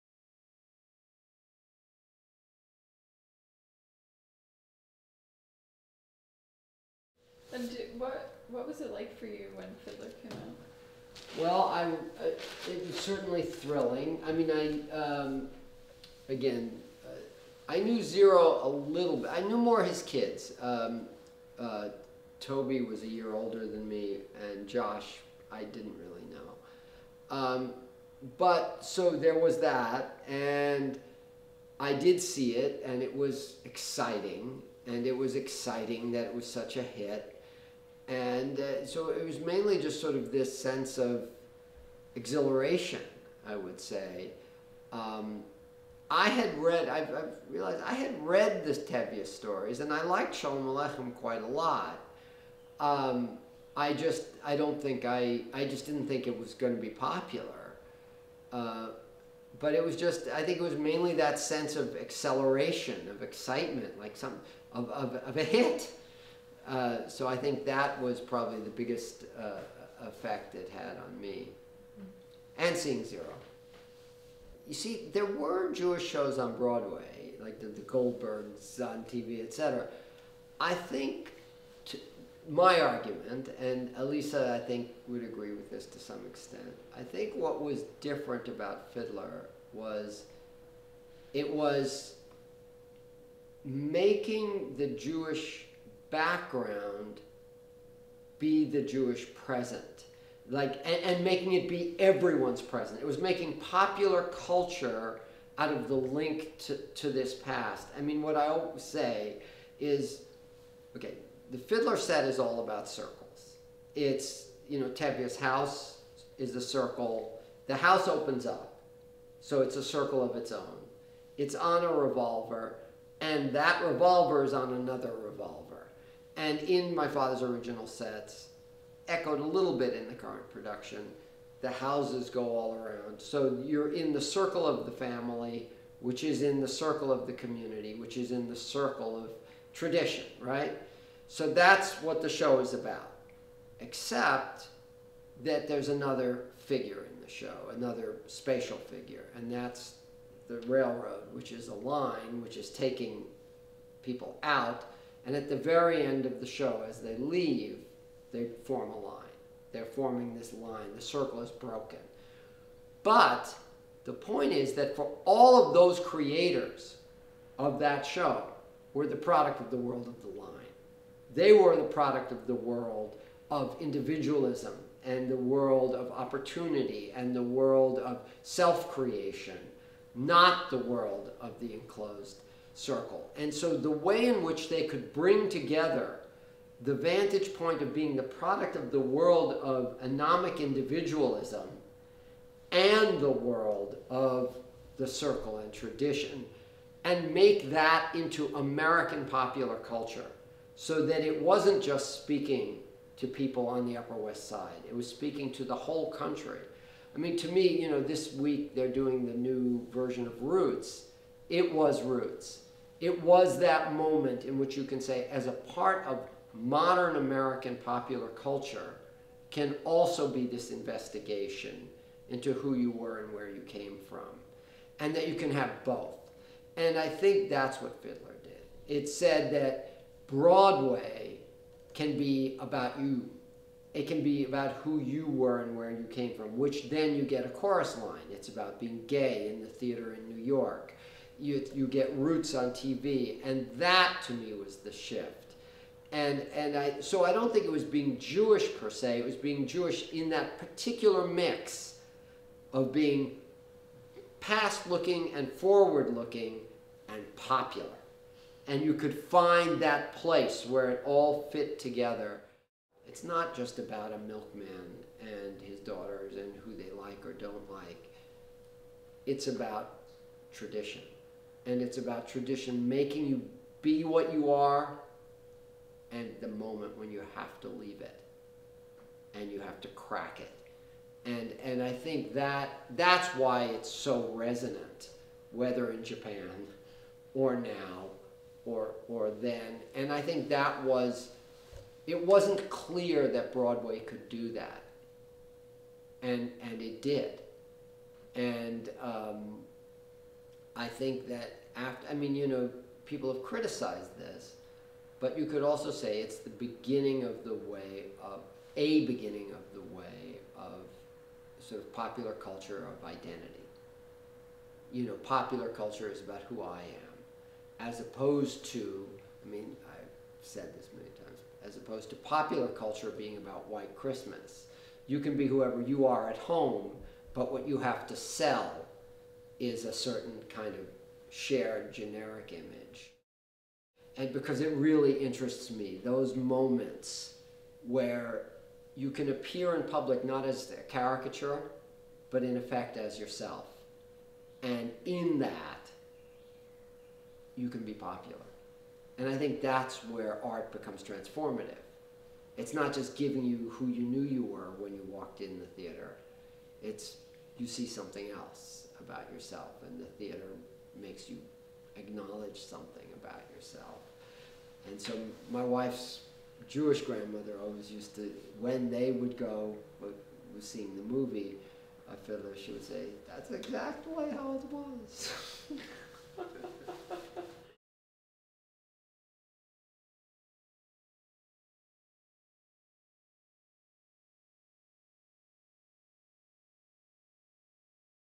The Universality of Fiddler on the Roof and Its Revolutionary Impact on Broadway
Marc Aronson, son of Yiddish theater set designers Boris and Lisa Aronson, discusses his reaction to Fiddler on the Roof's success, its influence on Broadway, and its universal messages about tradition, modernity, and identity.